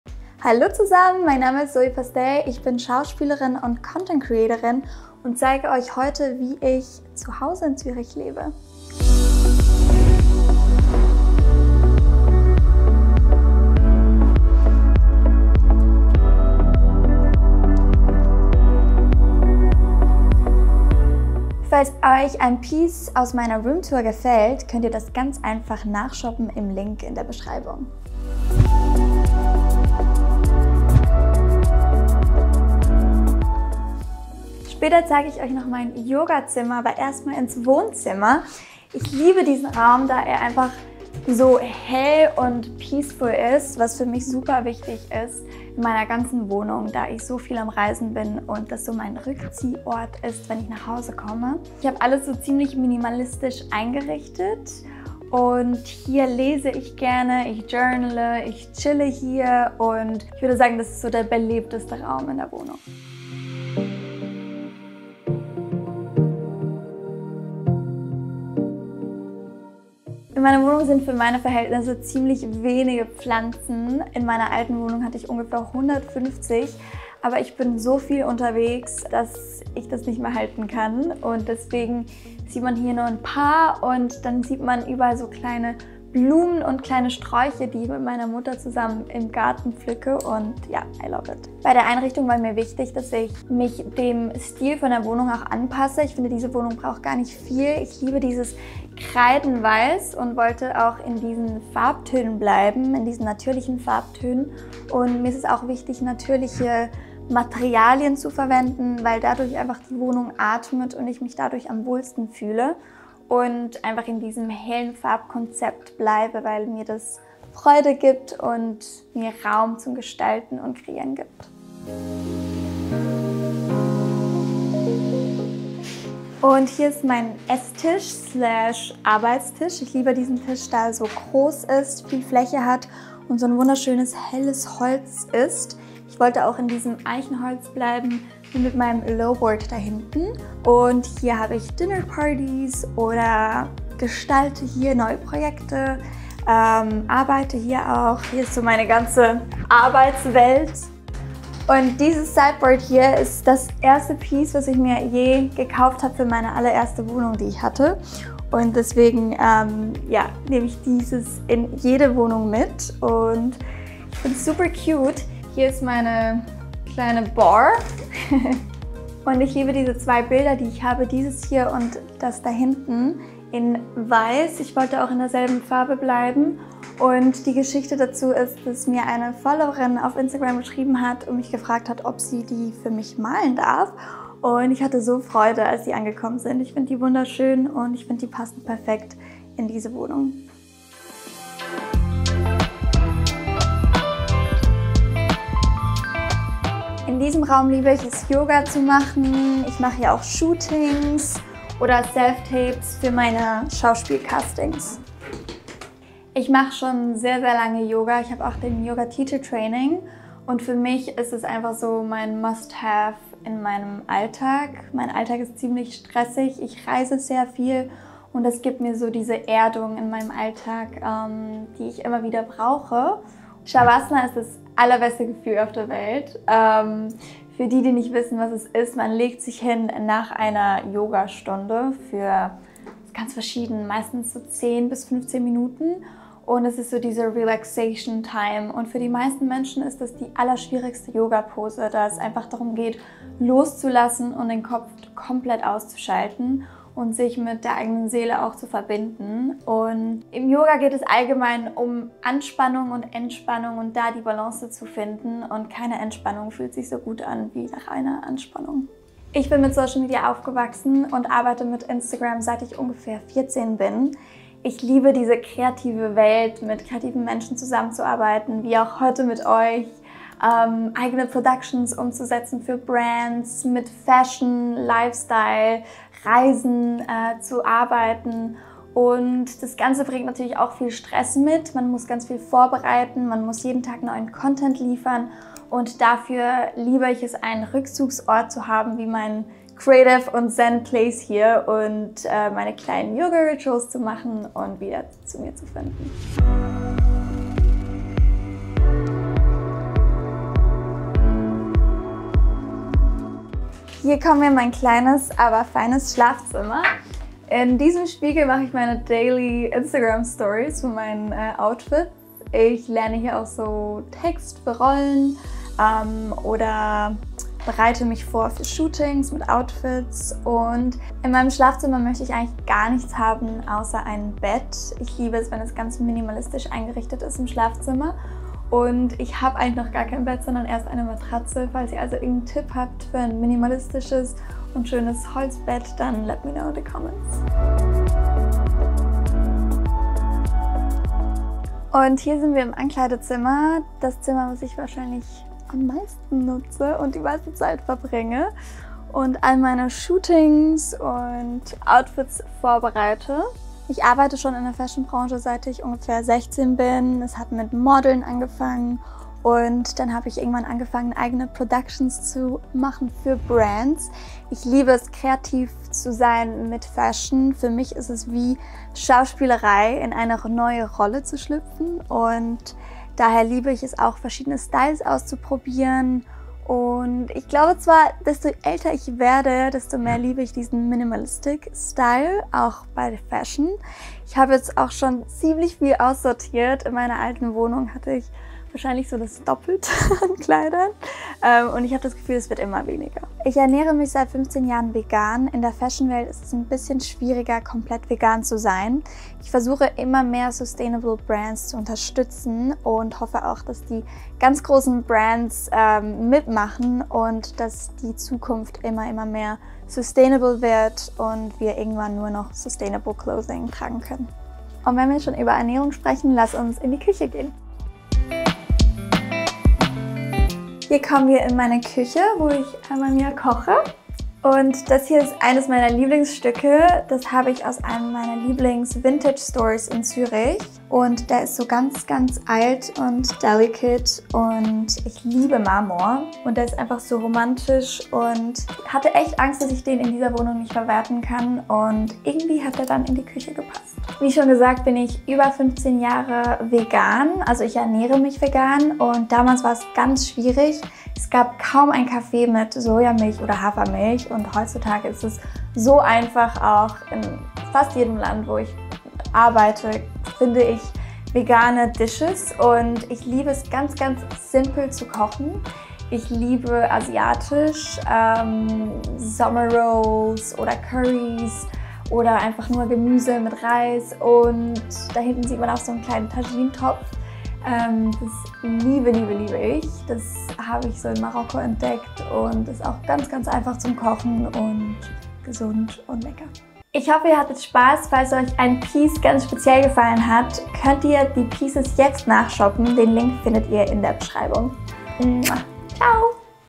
Altbauwohnung in der Schweiz | So traumhaft schön wohnt Zoë (Roomtour)
Heute dürft Ihr Euch auf eine Roomtour durch eine wunderschöne Altbauwohnung in der Schweiz freuen! Hier hat die Schauspielerin und Content Creatorin Zoë, die beruflich viel unterwegs ist, ihr Zuhause gefunden und entspannt gerne nach anstrengenden Drehtagen beim Yoga.  So traumhaft schön wohnt Zoë Pastelle!

Unsere Zoë Pastelle Roomtour starten wir im Wohnzimmer ihrer Züricher Altbauwohnung. Die aufwendigen Wandvertäfelungen stechen einem dabei sofort ins Auge. Beim Wohnzimmer Einrichten hat sich die Schauspielerin bewusst für helle Farben und natürliche Materialien entschieden, die sich besonders harmonisch in den Raum einfügen.

Beim Schlafzimmer Einrichten ihrer Altbauwohnung hat Zoë vor allem zu modernen Elementen gegriffen, die sie allerdings mit alten Stücken, wie beispielsweise ihrem großen Goldspiegel kombiniert. Auf diese Weise ist es ihr gelungen, ihren Altbau modern einzurichten, ihm aber dennoch eine besonders elegante Note zu verleihen.

Im Zoë Pastelle Interview verrät uns die Schauspielerin, wie sehr sie es liebt sich beruflich kreativ auszuleben. Schon als Kind stand Zoë als Model vor der Kamera, mittlerweile plant die heute 23 Jahre alte Schweizerin selbst Produktionen oder schlüpft für ihren Job als Schauspielerin immer wieder in neue Rollen. Abseits vom Medienrummel, ist Zoë Pastelles Leidenschaft das Yoga machen. Hierbei gelingt es ihr am besten sich zu erden und neue Energie zu tanken.

Der letzte Raum unserer Altbau Tour ist Zoës Küche. Hier hat sie sich für einen alten Vintage Tisch mit aufwendiger Verzierung entschieden. Beim Einrichten ihrer Altbauküche greift die Schauspielerin besonders gern zu verspielten Möbeln und Dekostücken im Landhausstil, um dem Raum einen besonderen Charakter zu verleihen.

So traumhaft schön wohnt Zoë in ihrer Altbauwohnung in der Schweiz! Wie haben Euch ihre Ideen zum Altbau einrichten gefallen?

Videoinhalt:
0:00 Intro
0:34 Wohnzimmer
2:02 Essbereich
3:14 Yoga Raum
5:01 Schlafzimmer
5:38 Ankleidezimmer
7:00 Küche